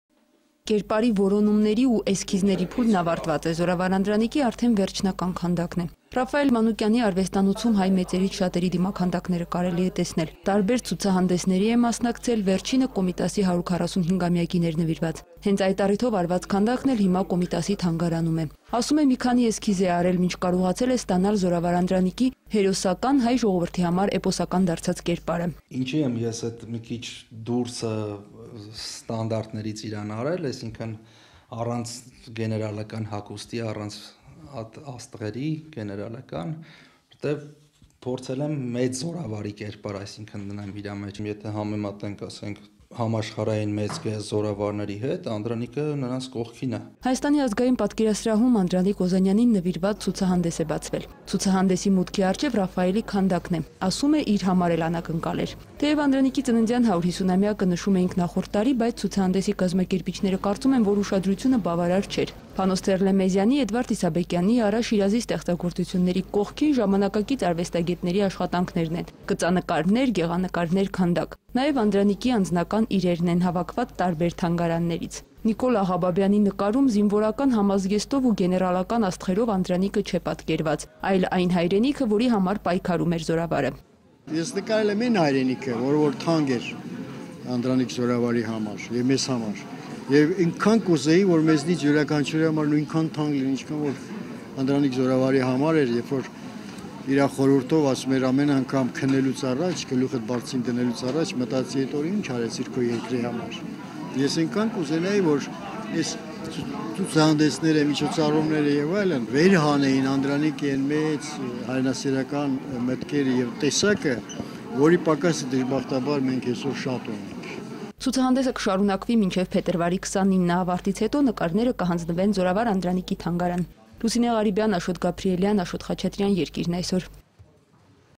Ազգային պատկերասրահում Անդրանիկ Օզանյանին նվիրված ցուցահանդես է բացվել
Ցուցահանդեսին ներկայացված է շուրջ 65 ստեղծագործություն. գեղանկար, գծանկար, քանդակ: